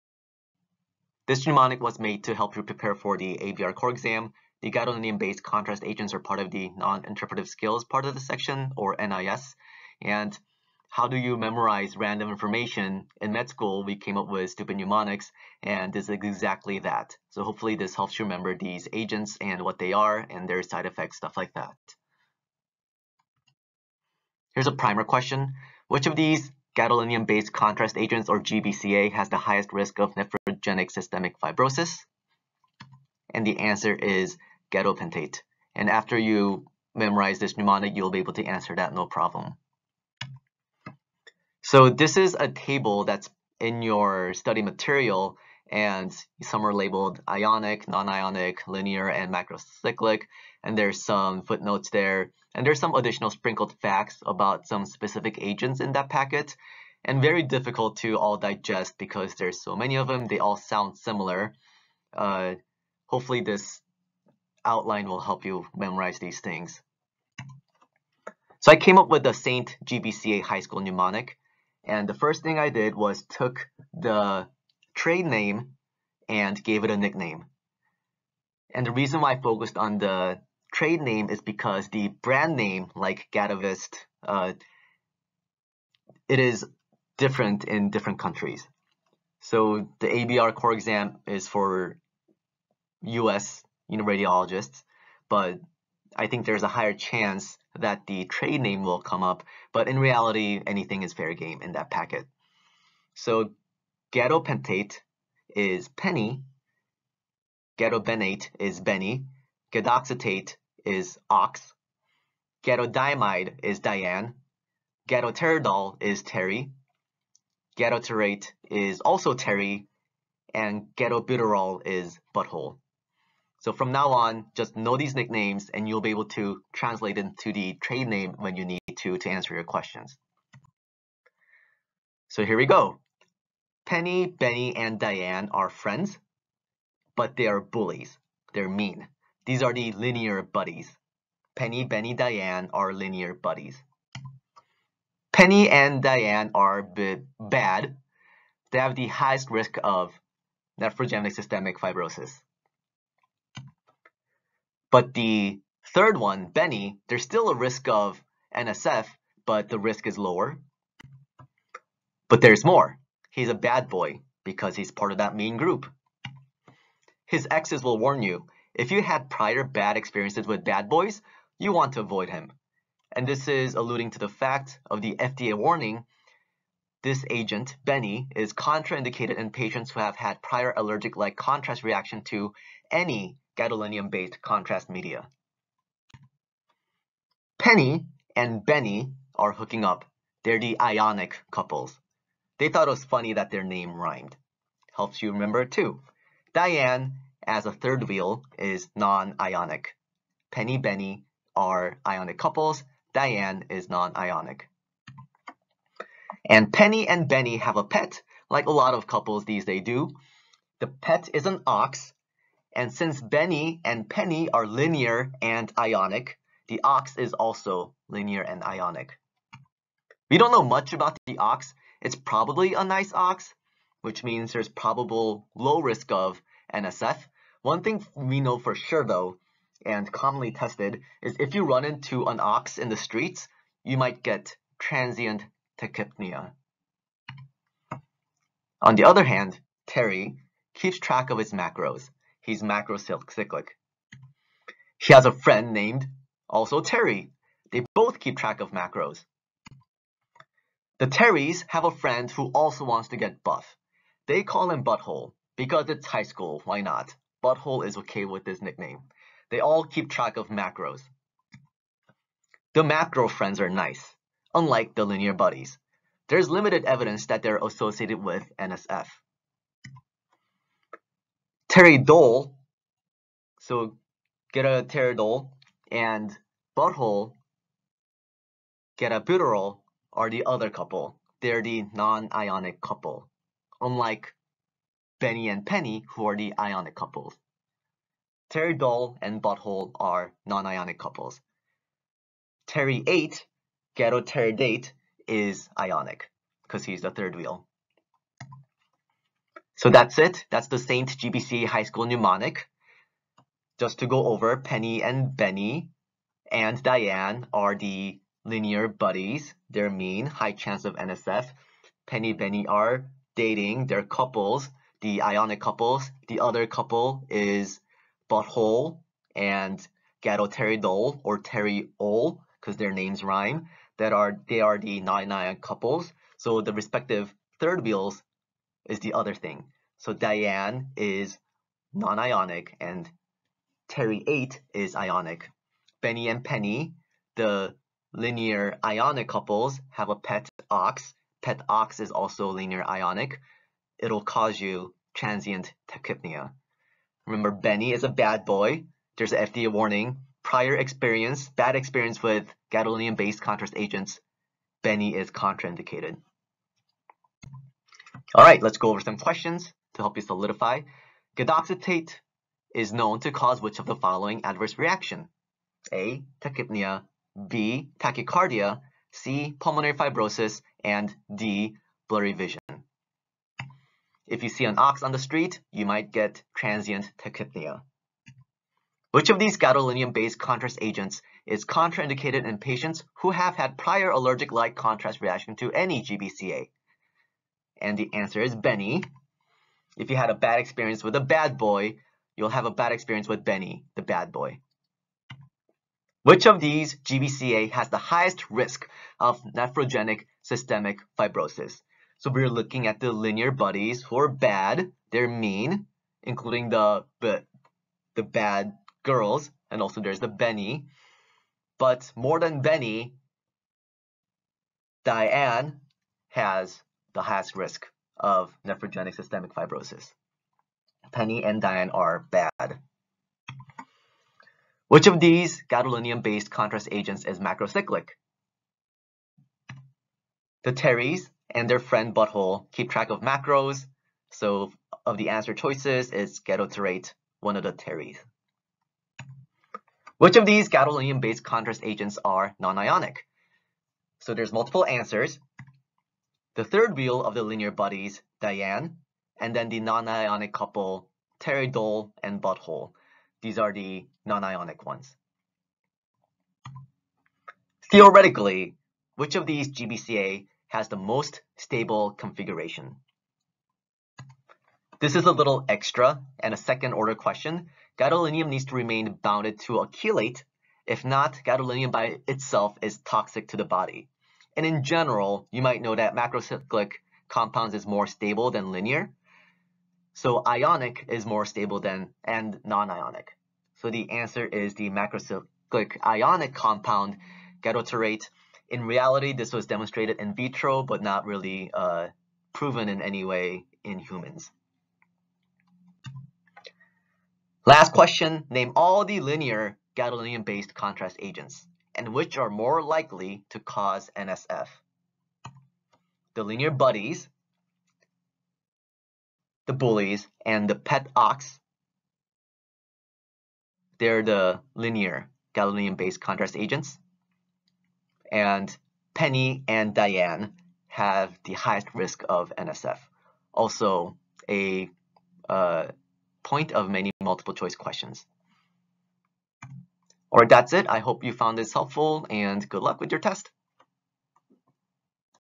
Memorize the Gadolinium Based Contrast Agents (St. GBCA High School mnemonic) | ABR Core Exam
Memorize your gadolinium-based contrast agents for the ABR Core exam with this dumb mnemonic. Based on the information in the 2024 Noninterpretive Skills (NIS) study guide.

👪 WHO ARE WE? Short version:
-Sam is a diagnostic radiology resident at Mayo Clinic - Rochester.